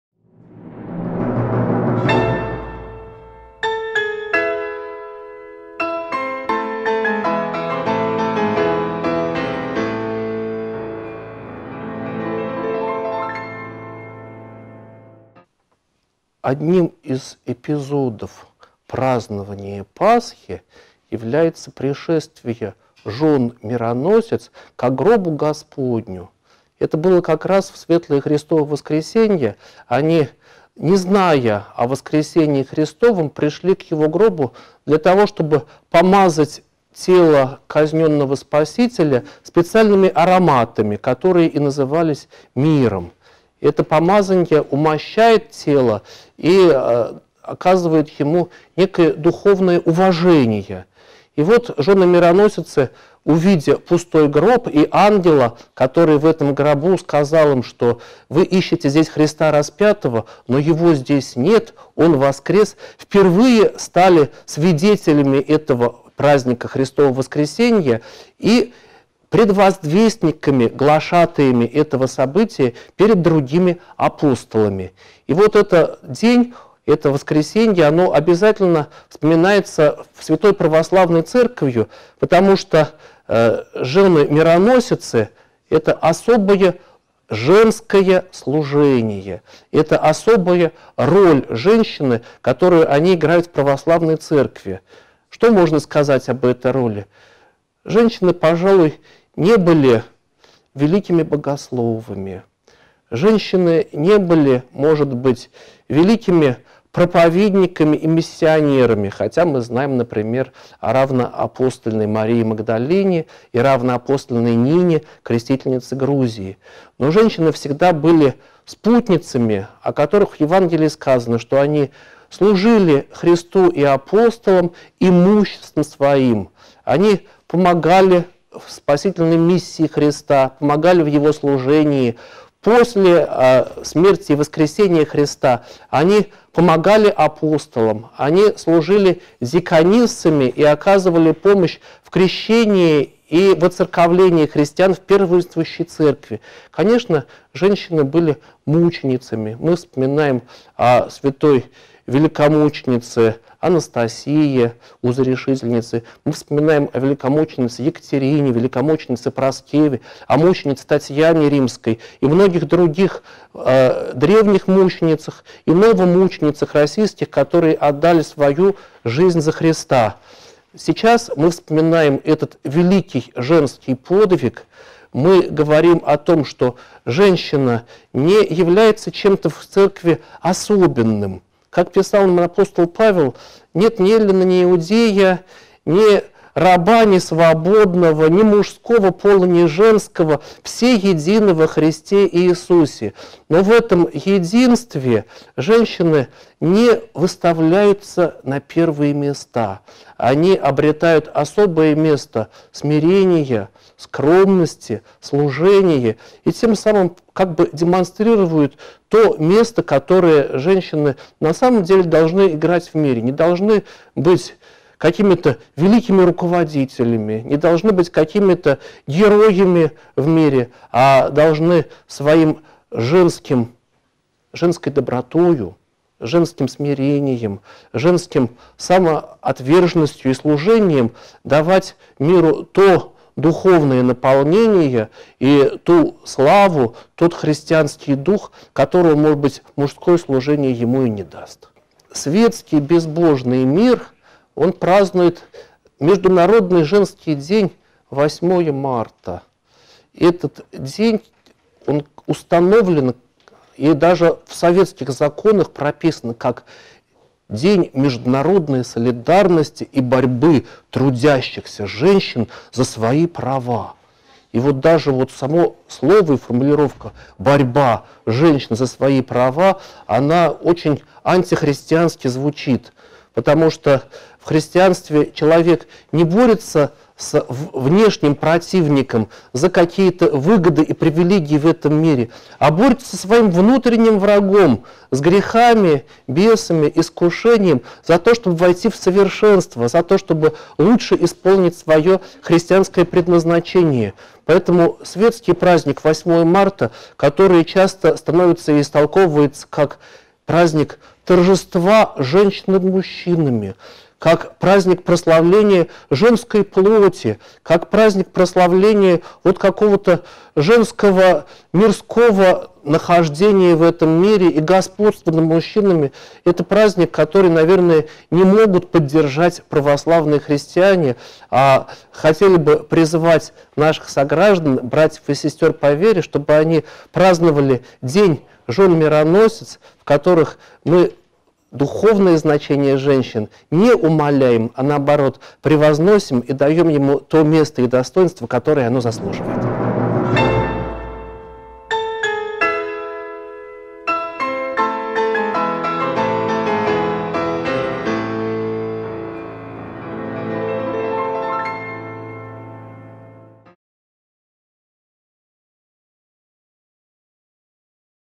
Вопросы о религии. Праздник Святых жен мироносиц
Есть мнение, что православной альтернативой 8-му марта является праздник Святых жён мироносиц, который отмечается в это воскресенье, 12 мая. Можно ли так считать и почему? Что это за праздник?
Отвечает ключарь Спасо-Вознесенского кафедрального собора г. Ульяновска Дмитрий Савельев.